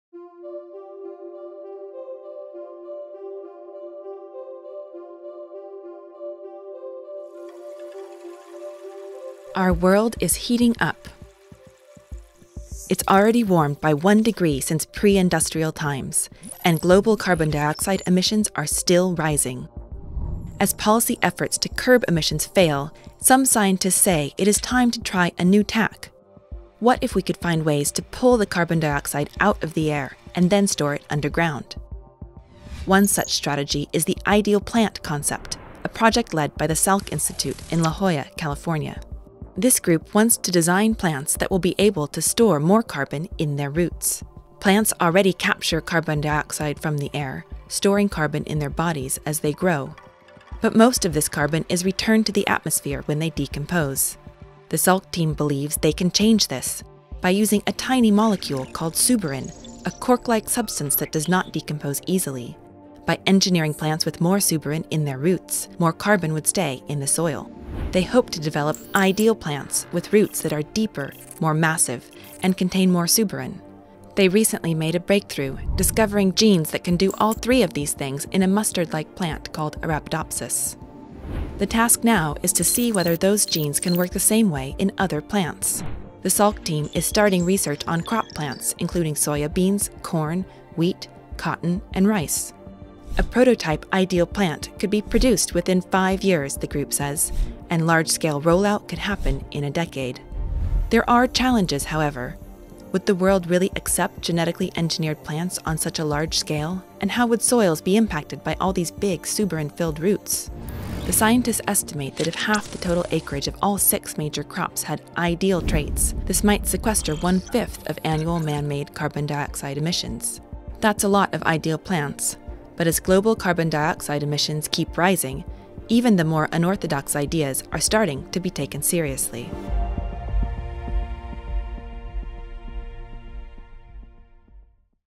Can a superplant save the planet? | Transformative Tech
As our world heats up, scientists are trying to design a superplant that can fight climate change by storing more carbon in its roots. It could eventually play a big part in curbing global warning.